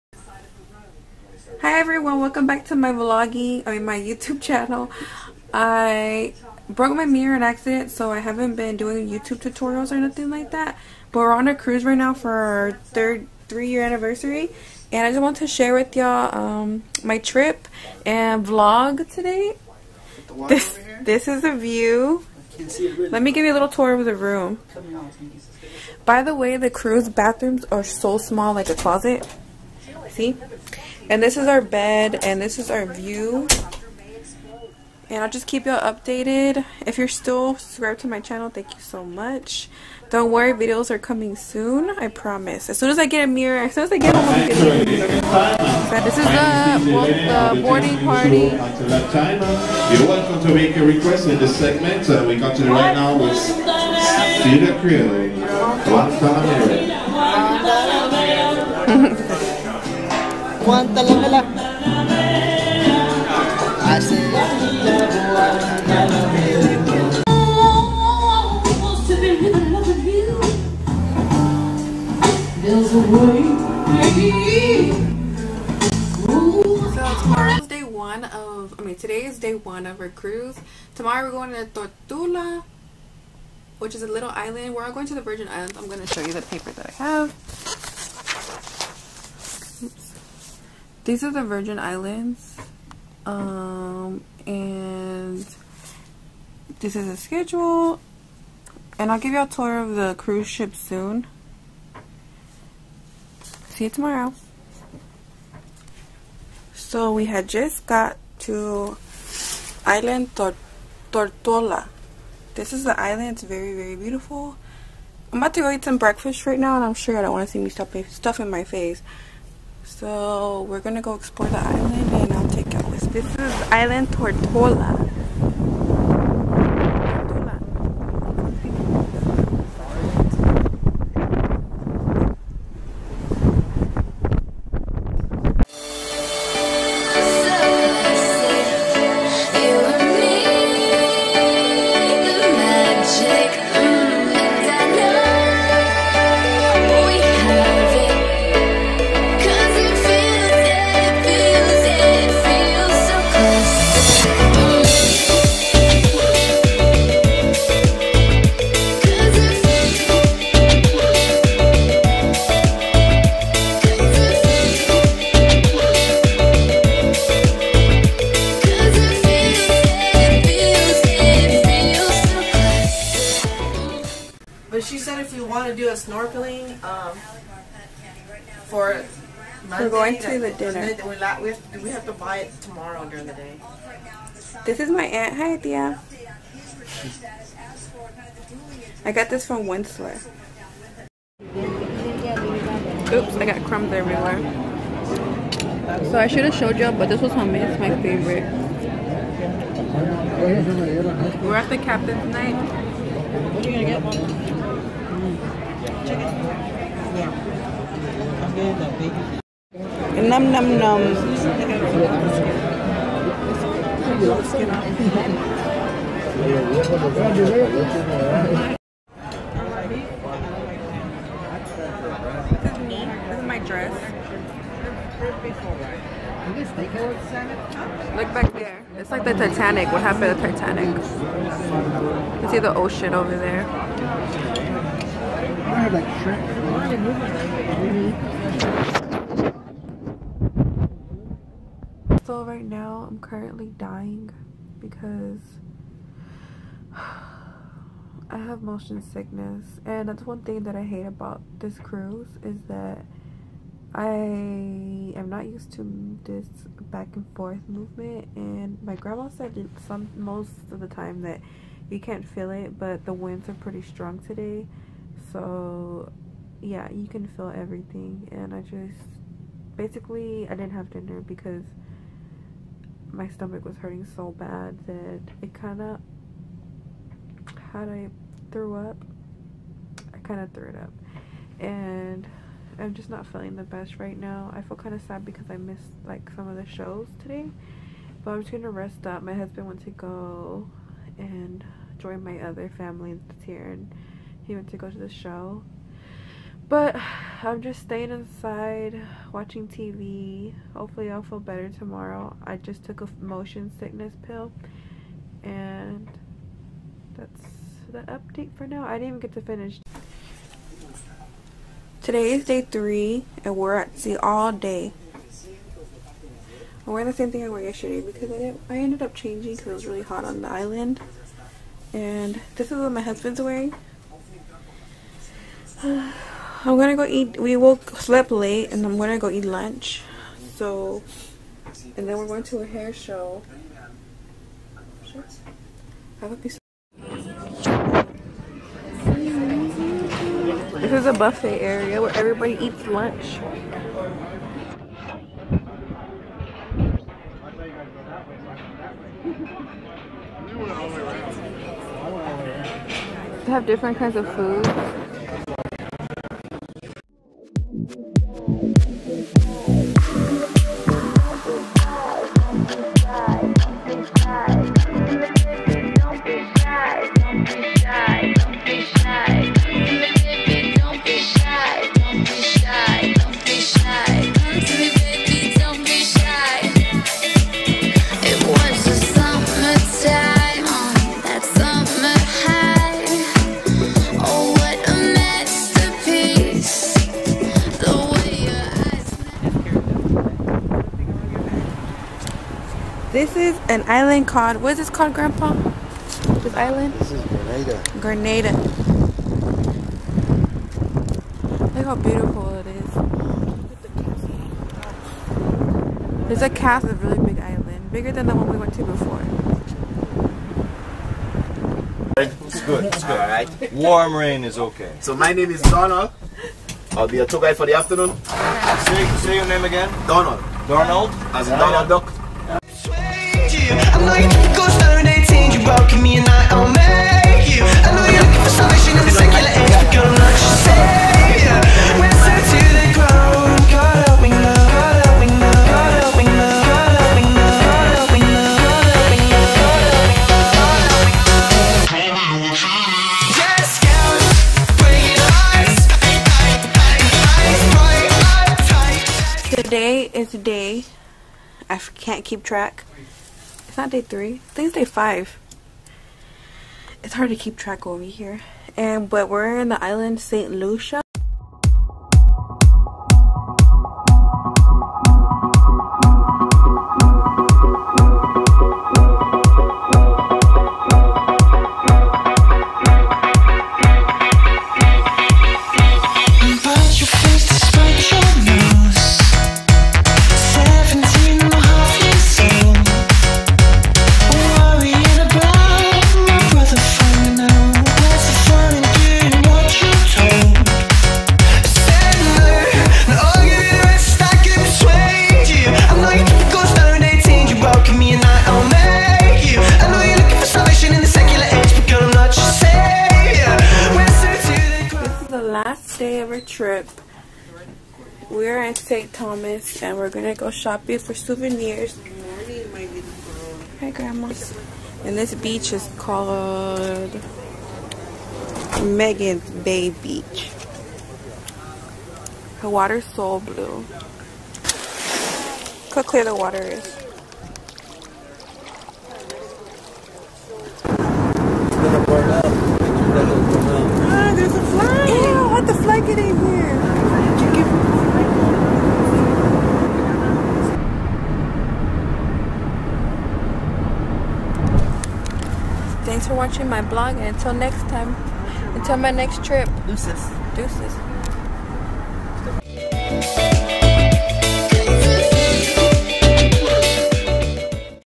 Crystal Lina Vlogs: Caribbean Islands
Hey everyone! Sorry I have been away for so long. Working on a better setup for my makeup tutorials. I miss youtubing so bad. My husband and I went on a cruise with my grand parents and aunt and I wanted to share my experience with everyone. I made the vlog sweet and fast ( a little too fast on some parts) but I will be back very soon. Thank you for sticking with me. xoxo